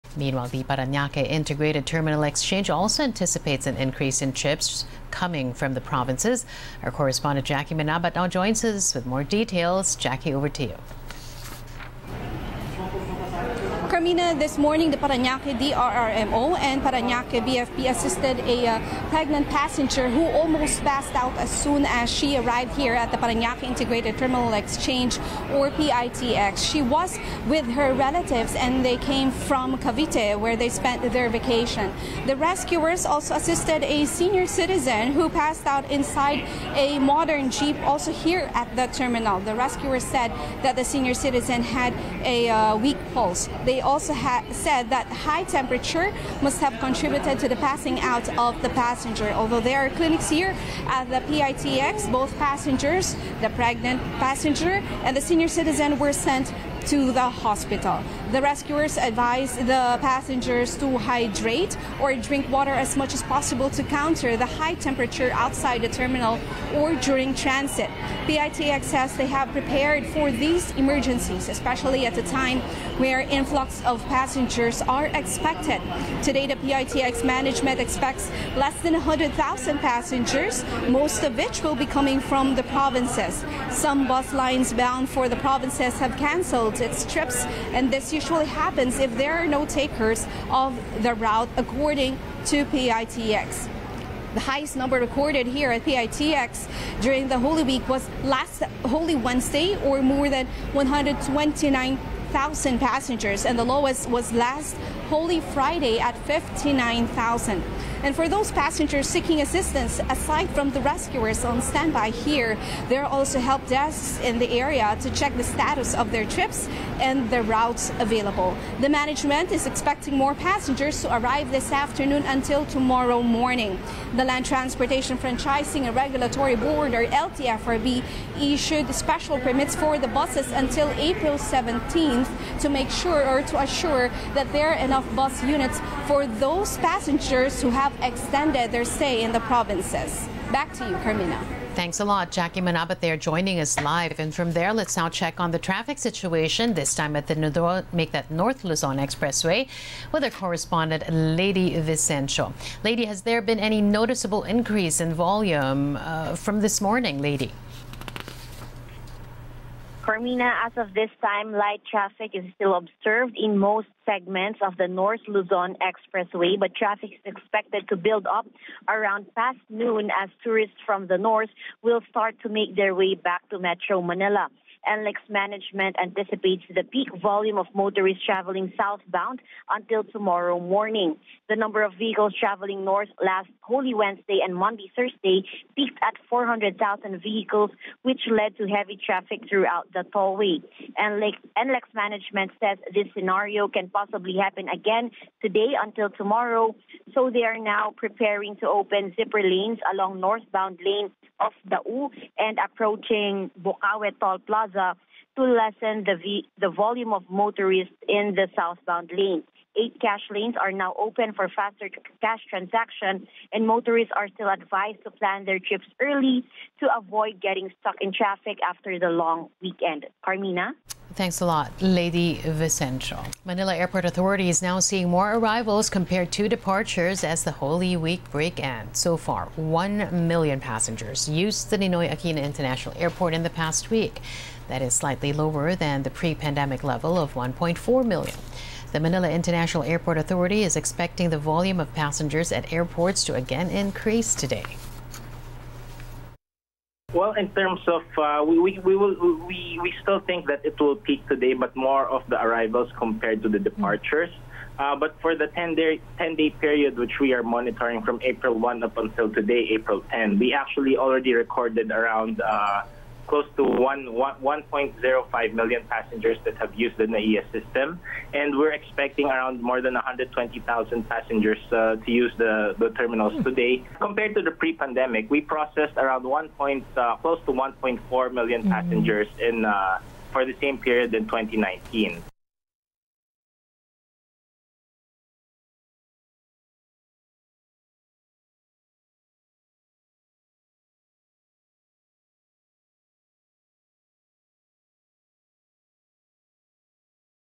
PITX expects increase in trips coming from provinces after holy week break | ANC
Dateline Philippines: Jacque Manabat reports.